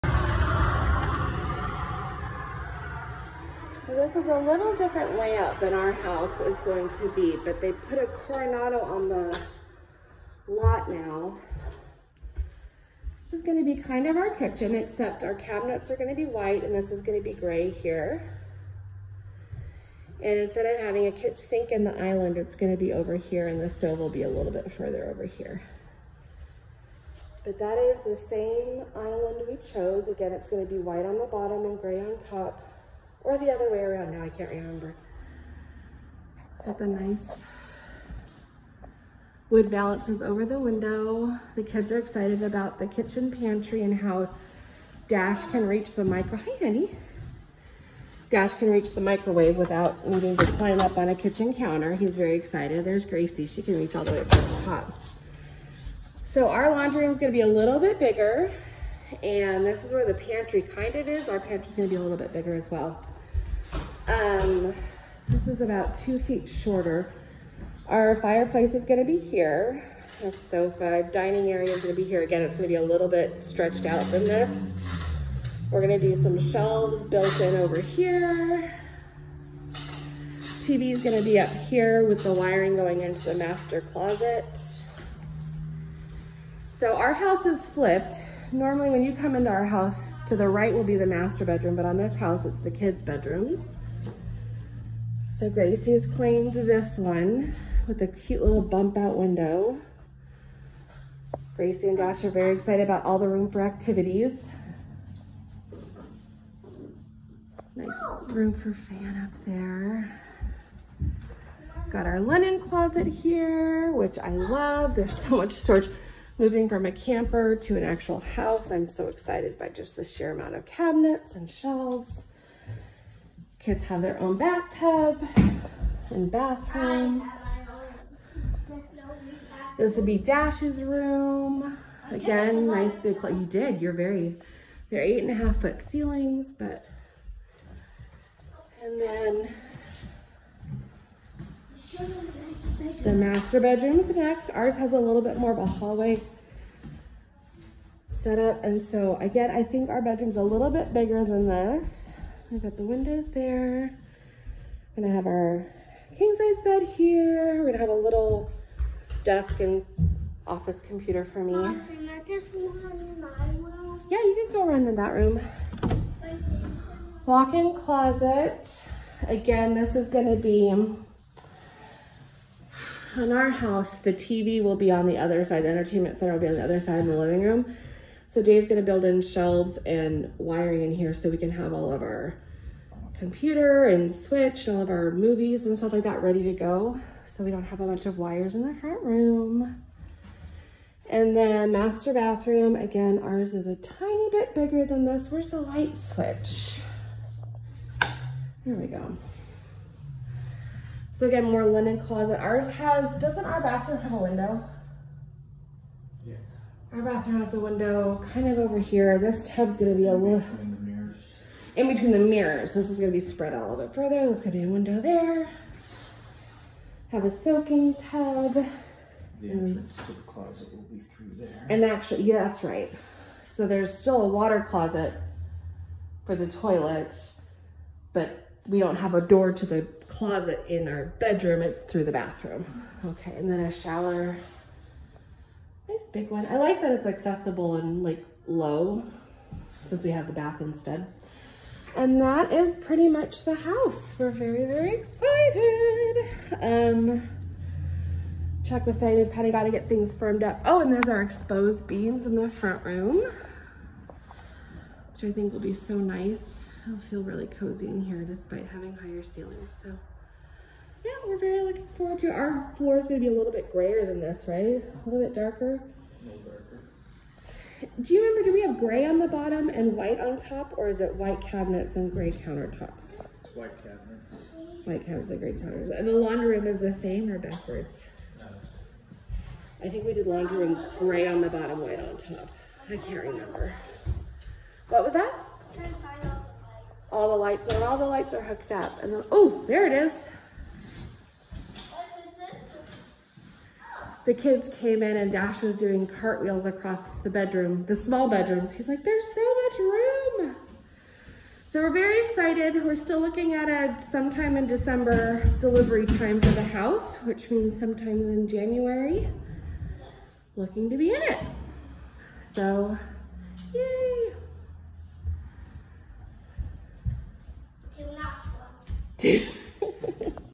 Walk through the Coronado by Clayton Homes
Had to swing by El Dorado Homes to sign off on some last details for our manufactured home and they had a Coronado on the lot! Ours is a little bit bigger and the floor plan is flipped but it really got the kiddos excited! Still planning on a December delivery! 🤞🏻